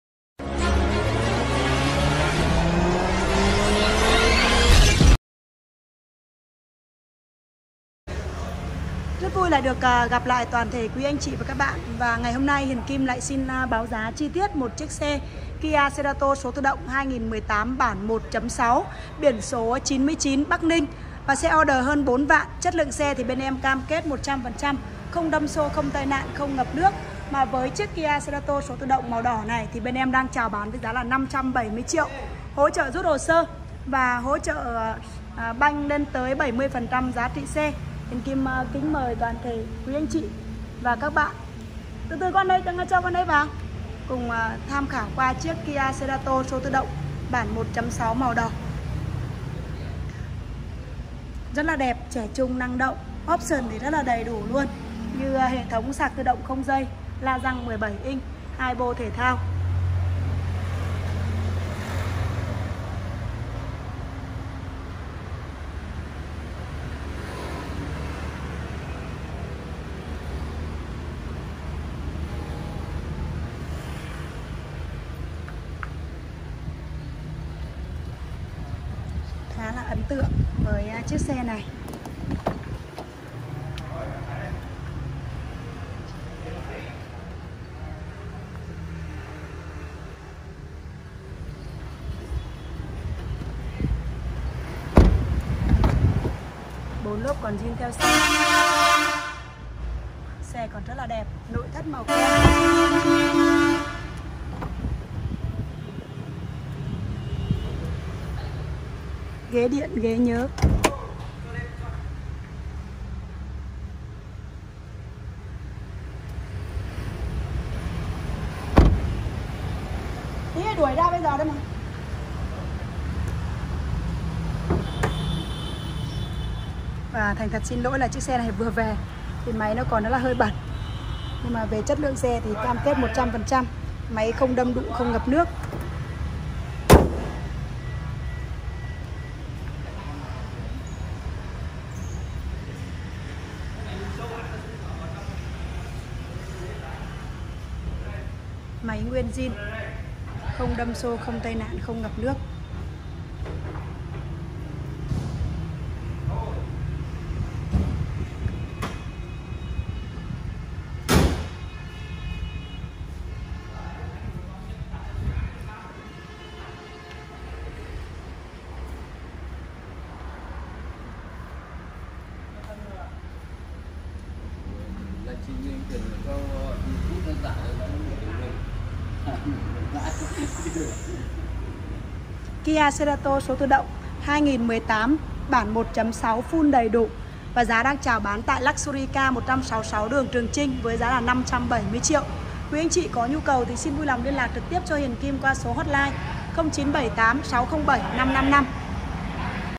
💎 Kia Cerato 1.6AT sx 2018 ODO hơn 3v km. Giá 568tr LH: 0828.066.166 - 0978.607.555
Kia Cerato 1.6AT sx cuối 2018 số tự động, đẹp xuất sắc trang bị nhiều option: Màn hình cam lùi, cảm biến trước sau, điều hoà tự động hai vùng độc lập có cửa gió hàng ghế sau, ghế lái chỉnh điện nhớ 2 vị trí, vô lăng tích hợp phím điều khiển chức năng.
👉Giá 568tr
👉 Hỗ trợ bank 70% giá trị xe
Cam kết:
💯 Xe chưa từng bị tai nạn.
💯 Xe chưa từng bị ngập nước
💯 Xe chưa từng đại tu động cơ và hộp số.
💯 Đảm bảo giấy tờ hợp lệ và đủ điều kiện để mua bán và sang tên.